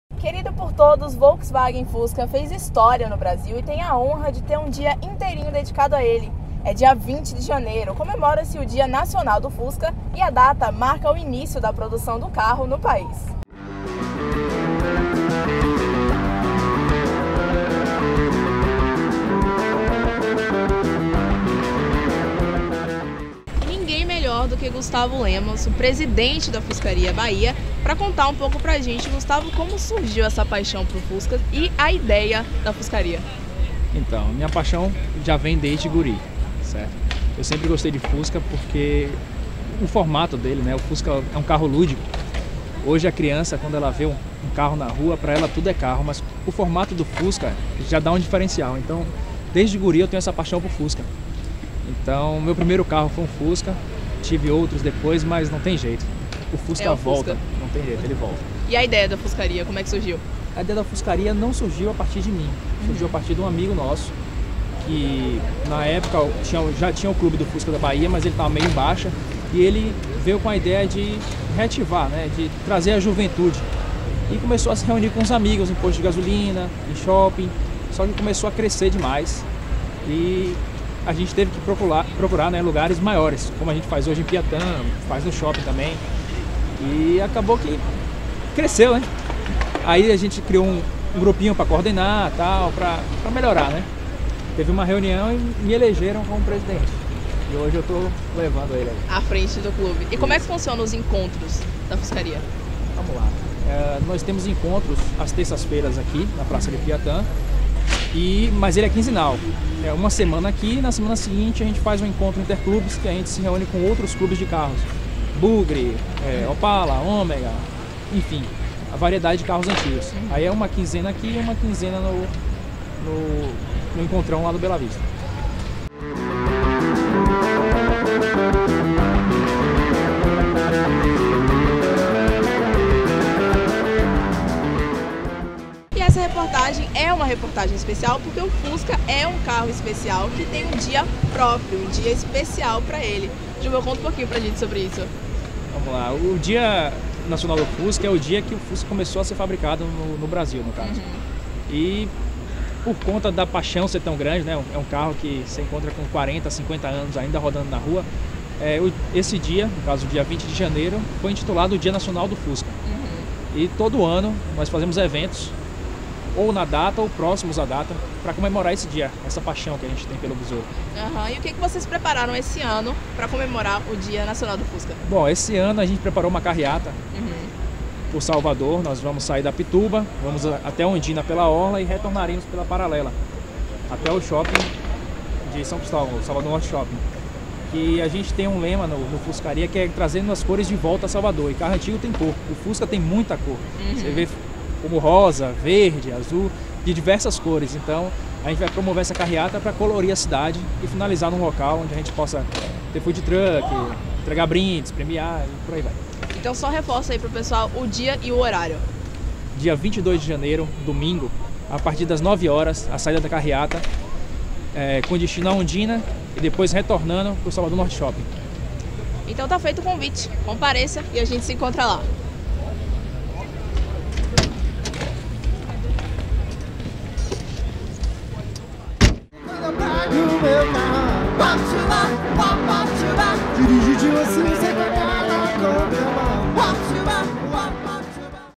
Dia Nacional do Fusca.
Dia 20 de Janeiro é comemorado o dia nacional do Fusca, por isso a nossa repórter Renata Ferrari, foi conversar com o Presidente da Fuscaria Bahia, sobre as comemorações em torno de um dos maiores ícones da industria automobilística.